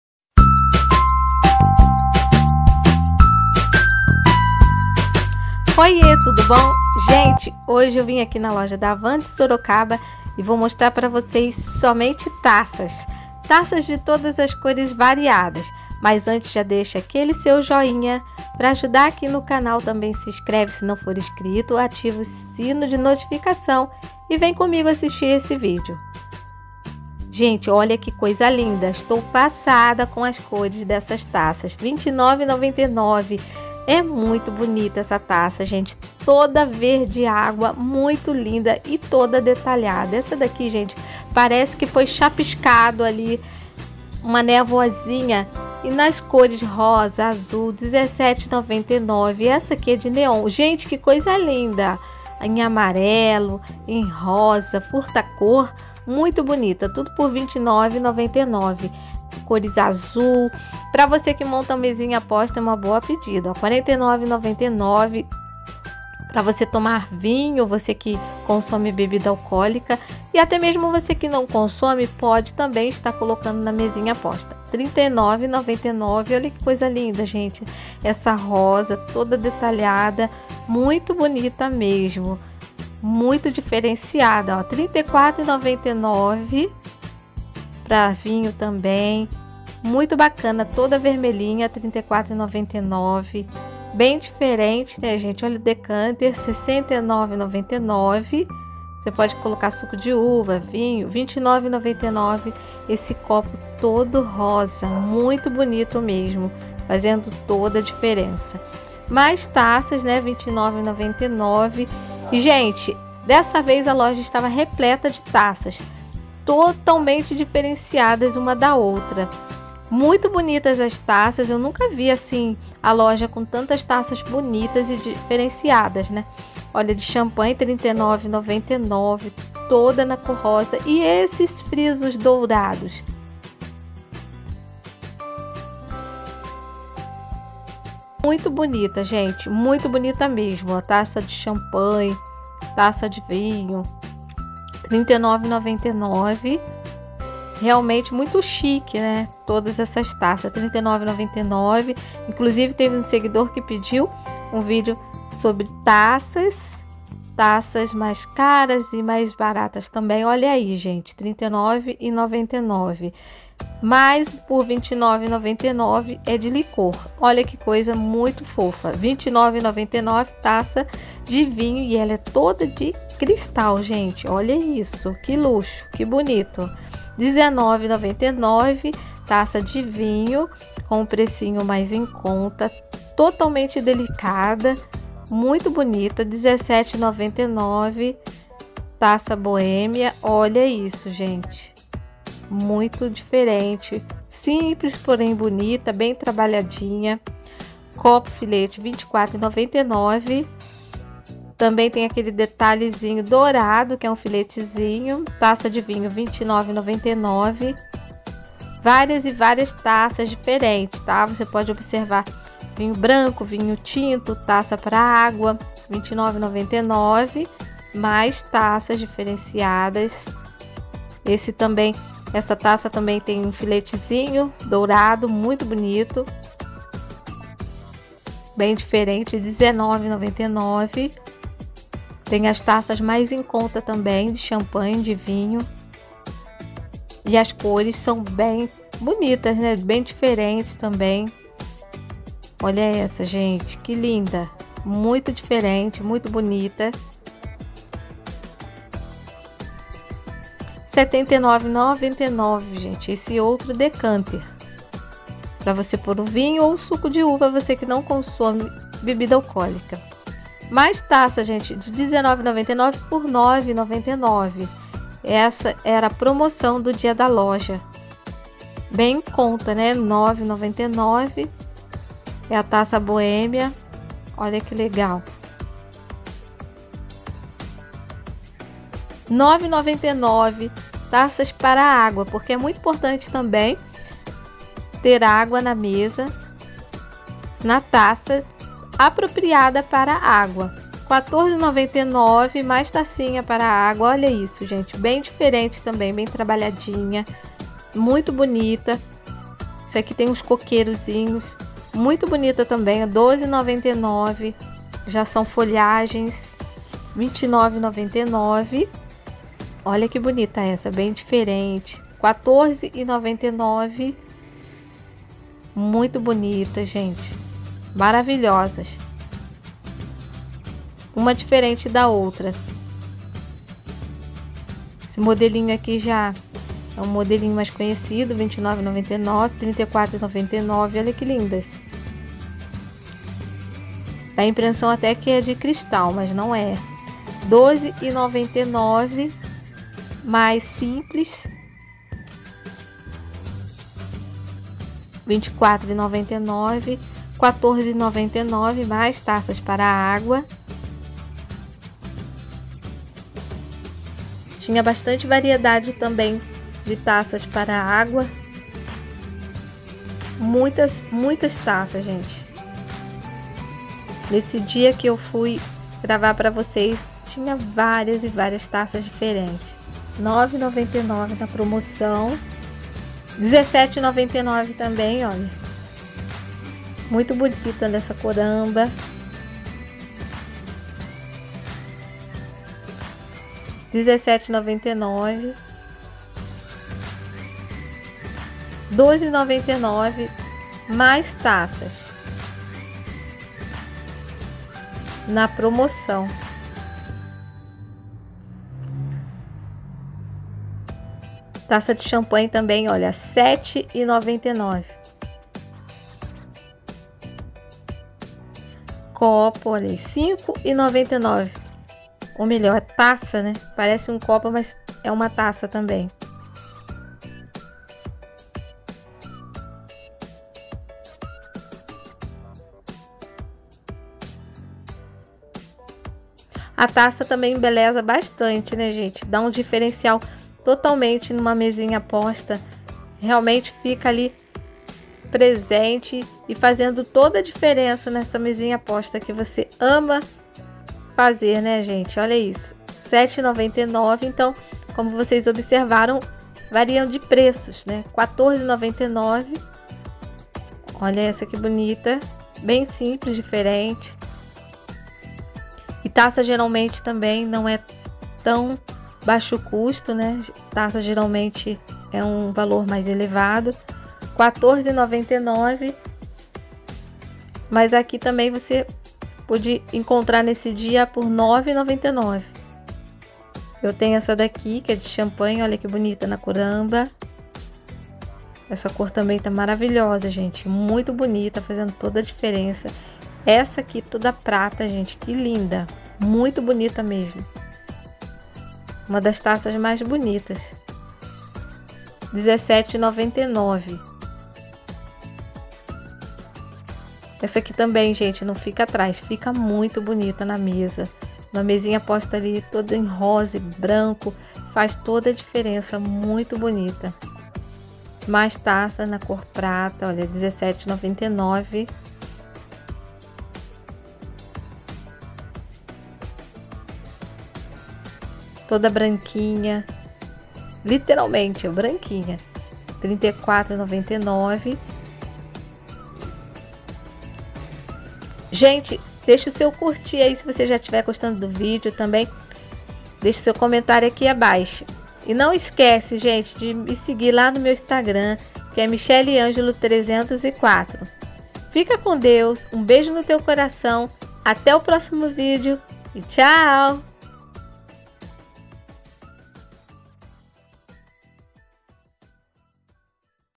SÓ TAÇAS PARA MONTAR MESAS POSTAS /HAVAM SOROCABA
Olá meus amores tudo bem?

Taças bonitas para montagem de mesinhas postas.

Loja Havan de Sorocaba SP